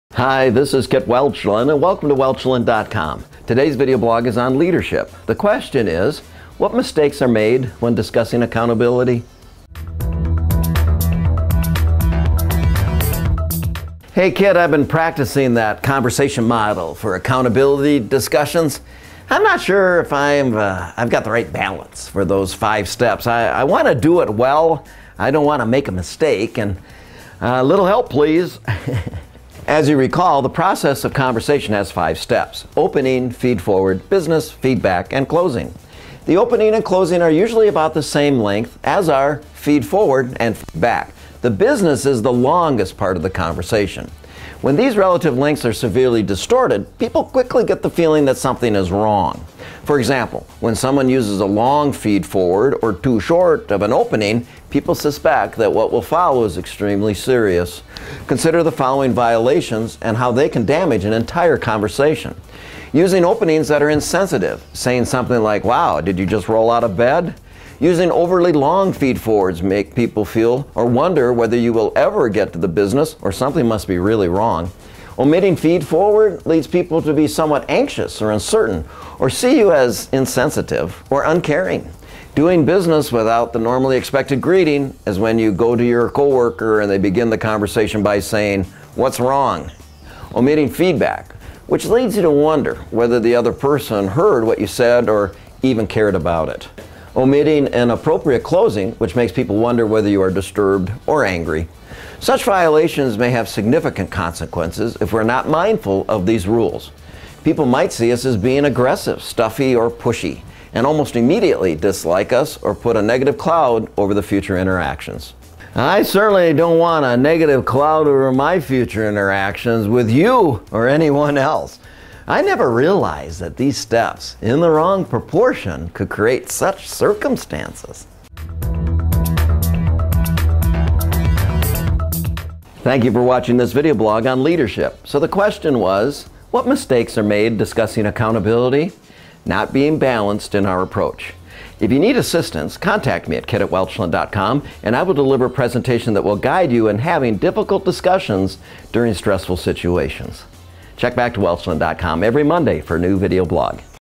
Leadership: What Mistakes Are Made When Discussing Accountability?
Having the right balance is important when having the conversation concerning accountability.

As you recall, the process of conversation has five steps: opening, feedforward, business, feedback, and closing.

The opening and closing are usually about the same length, as are the
feedforward and feedback. The “business” is the longest part of the
conversation.